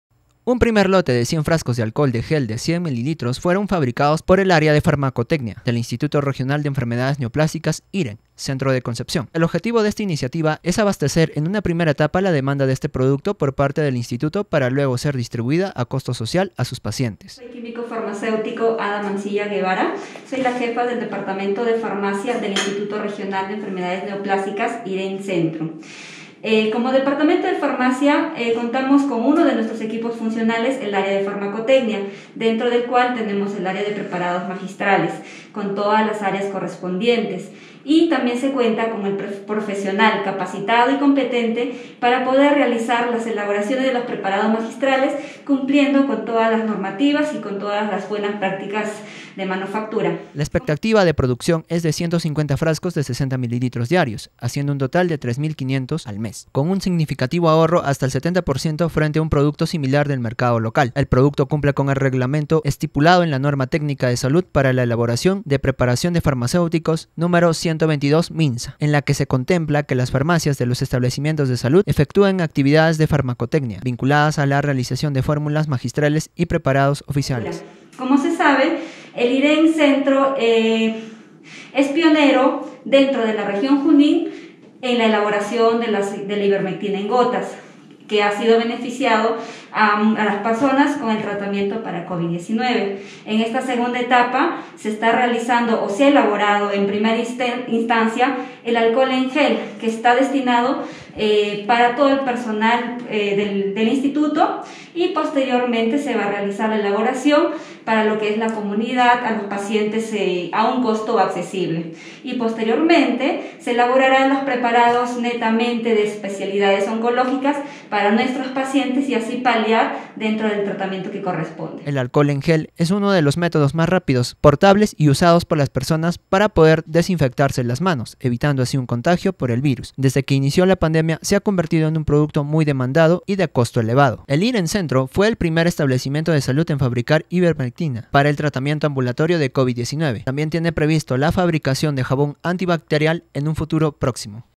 REGIONAL: IREN elabora su propio alcohol en gel | KDNA NOTICIAS
Un primer lote de 100 frascos de alcohol en gel de 60 mL. fueron fabricados por el área de farmacotecnia del Instituto Regional de Enfermedades Neoplásicas IREN Centro de Concepción.